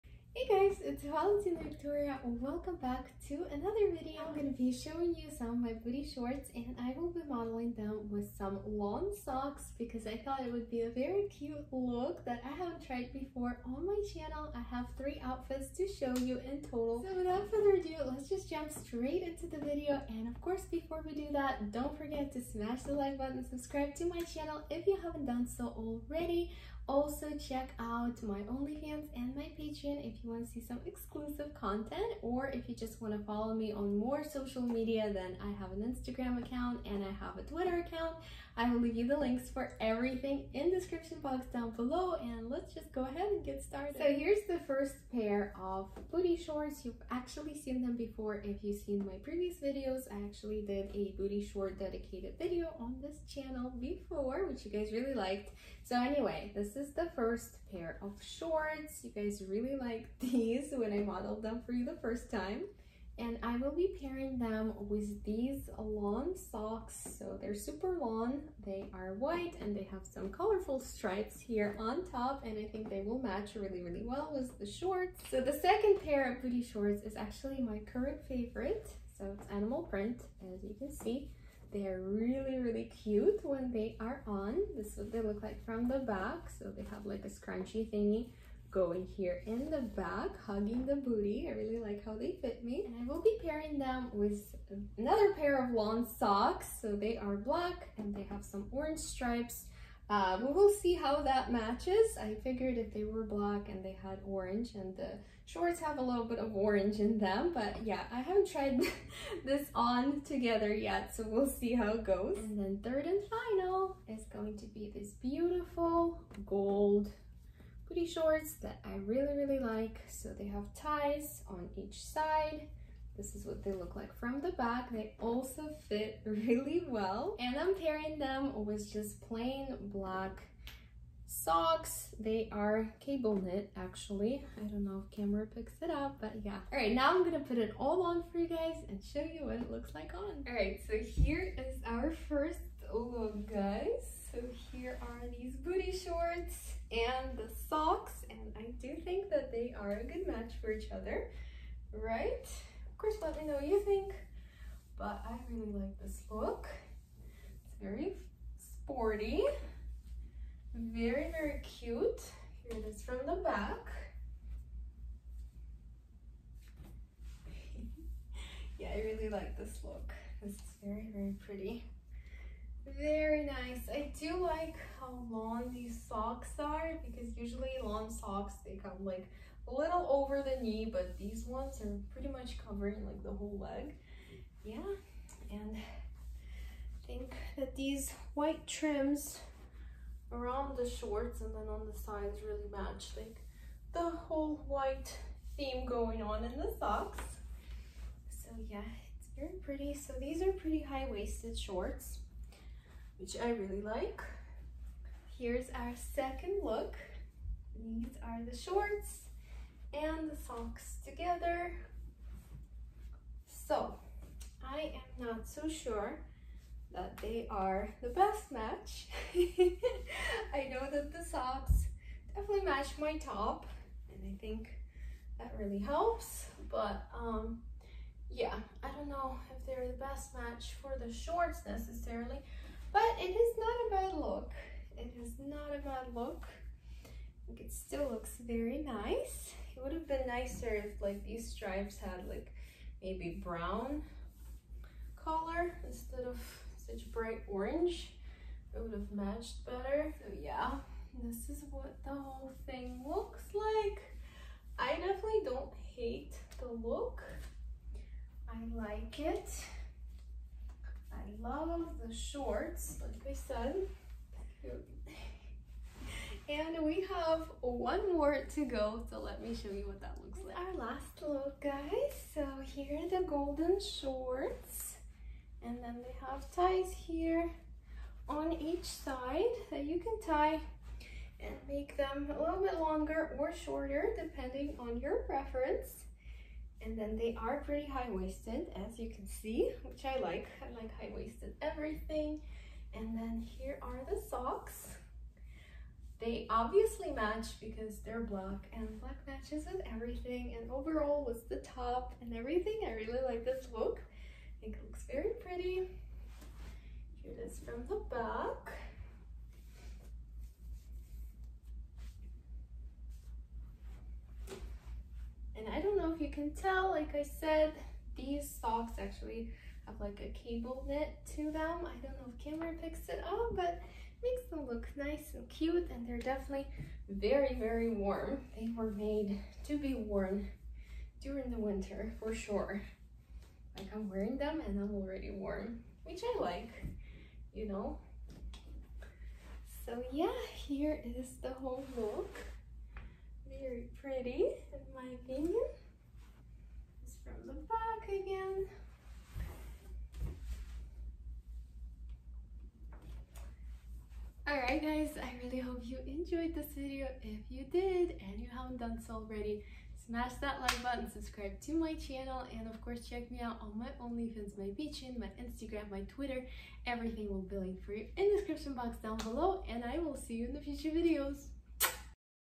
Long Socks and Booty Shorts Try On Haul
Hey guys! Trying on some long socks and shorts for you today! Enjoy:-)